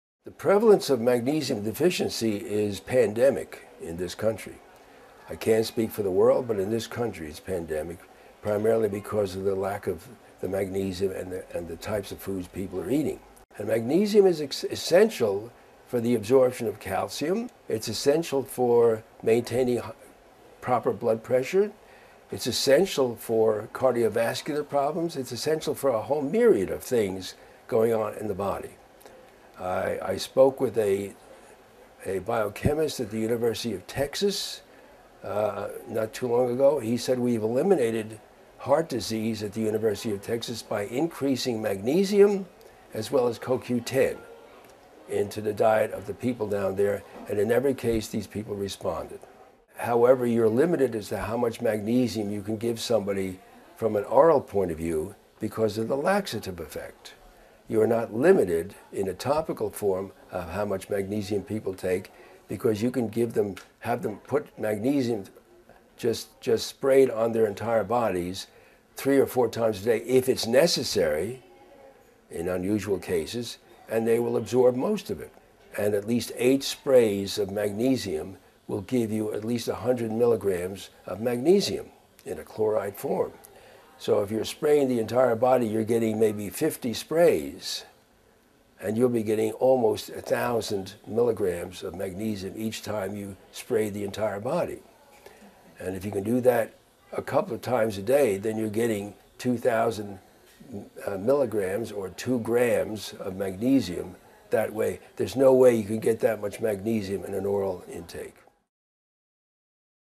Maintaining Cardiac Health with Magnesium
- Dr. McAuliffe explains the importance of magnesium in our significantly magnesium deficient society.

--- Ancient Minerals Ultra Pure Topical Magnesium ---

Dr. William McAuliffe discusses just how far we as a society have fallen into magnesium deficiency, and the difficulty of trying to reverse it through food alone.  Through his work and research, he's discovered a myriad of issues resolved through simply adding this crucial mineral to your system. From heart disease, to blood pressure, to the absorption of other nutrients, magnesium is essential to overall health.
However, as Dr. McAuliffe describes, adequate intake of magnesium via oral supplements is very difficult to achieve.